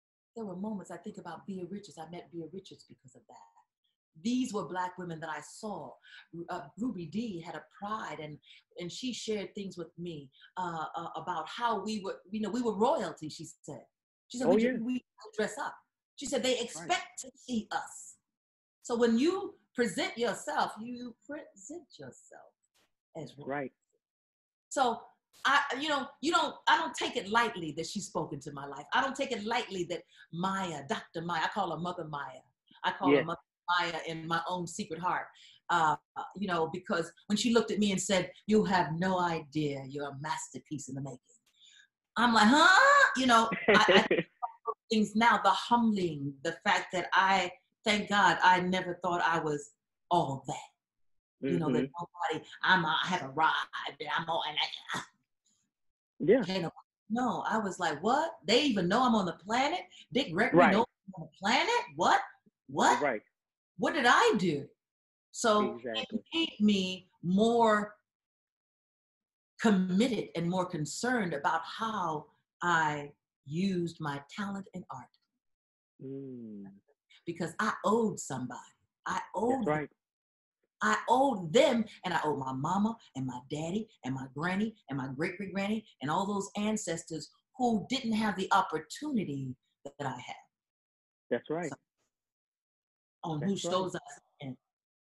Phyllis Yvonne Stickney Talks The Reason She Became Committed & Concerned w/ How She Used Her Talent
Check out this clip from my new interview with Actress/Comedienne Phyllis Yvonne Stickney for The Tony Holley Show! (Part 1 of the interview will be posted Sunday 9/27 at 8PM EST.)

In this clip Ms. Stickney talks about why she became committed & concerned with how she used her talent.

Phyllis Yvonne Stickney is an American film and television stand-up comedian and actress. She portrayed Wanda Shepard in Die Hard with a Vengeance.

Stickney's credits include the Spike Lee films Jungle Fever and Malcolm X (with Sharon Washington), What's Love Got to Do with It, How Stella Got Her Groove Back and New Jack City.

Comment who you would like to see me interview below!